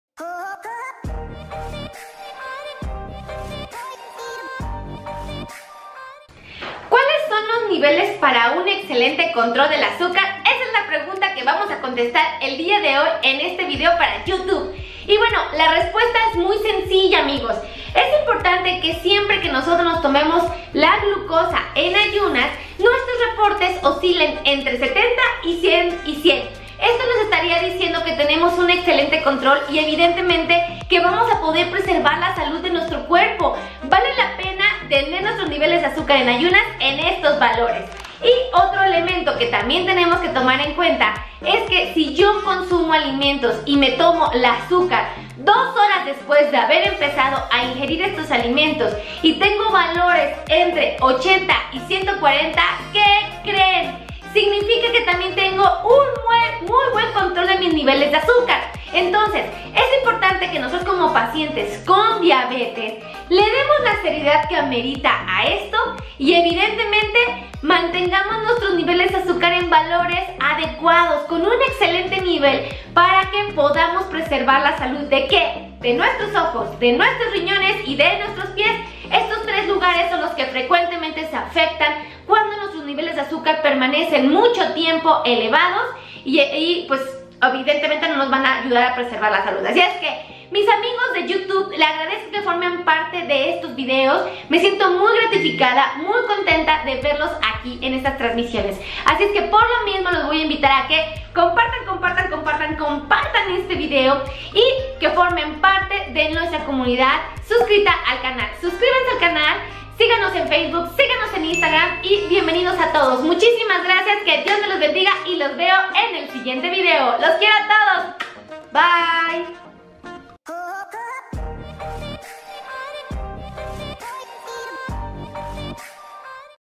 🟢¿COMO SE QUE MI AZUCAR ESTA BIEN? ☑✔⁄▶Dra. Tejeida Melissa
Consultas: 📞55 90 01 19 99📞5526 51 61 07  en el 🏢World Trade Center Ciudad de México
Consejos para el cuidado de pacientes que viven con diabetes